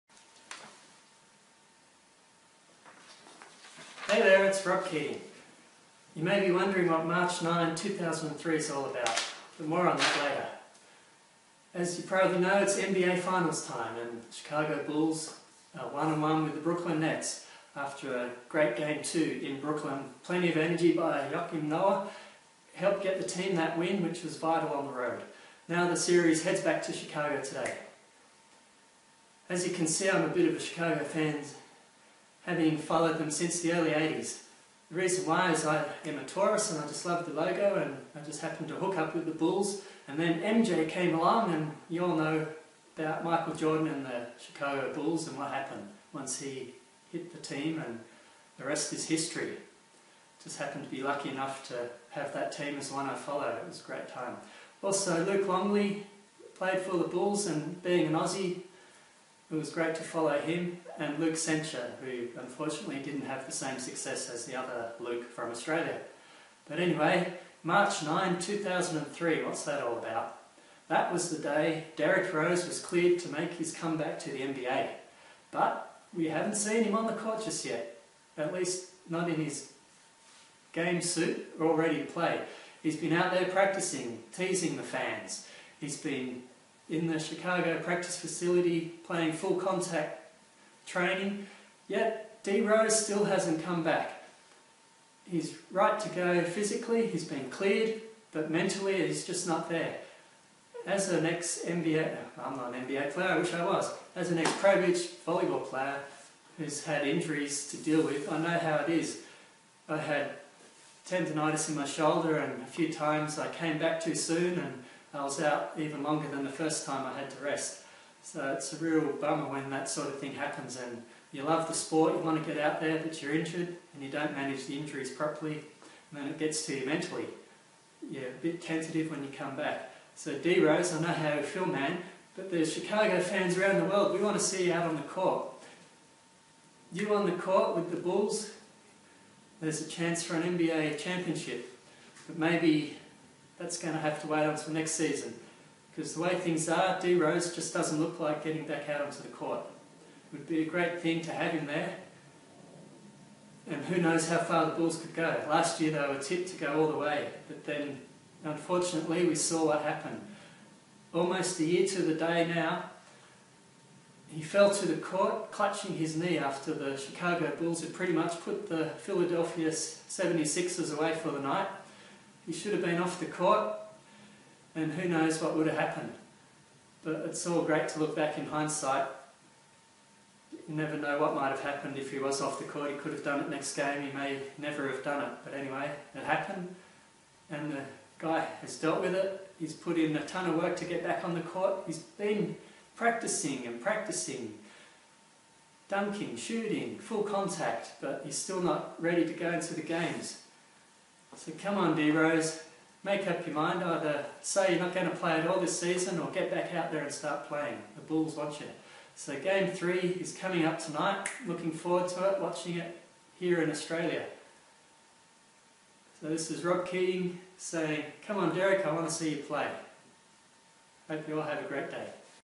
Derrick Rose comeback
March 9, 2013 is a big date for Chicago Bulls fans but it has been not what we expected since that time. I explain in this video.